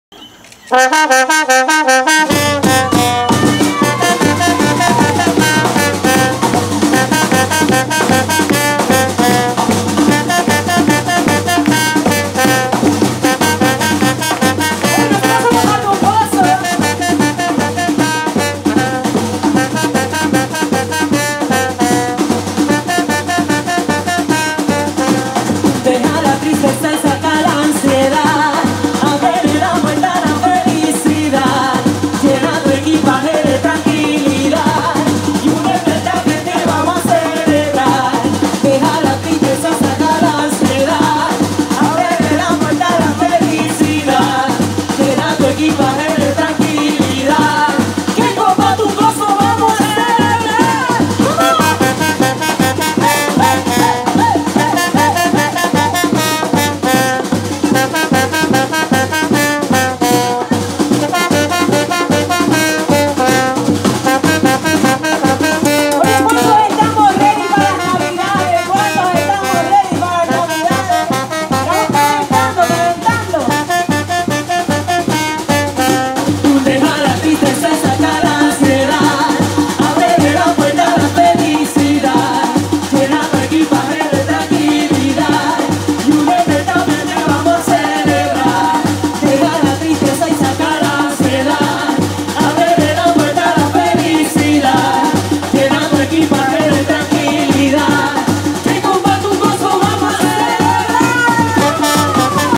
1ra Feria Agricola Familiar en Aguas Claras en Ceiba, BATUNGOZO BATUCADA CRISTIANA
Deja la trizteza saca la anciedad
1ra Feria Agricola Familiar en Aguas Claras en Ceiba Puerto Rico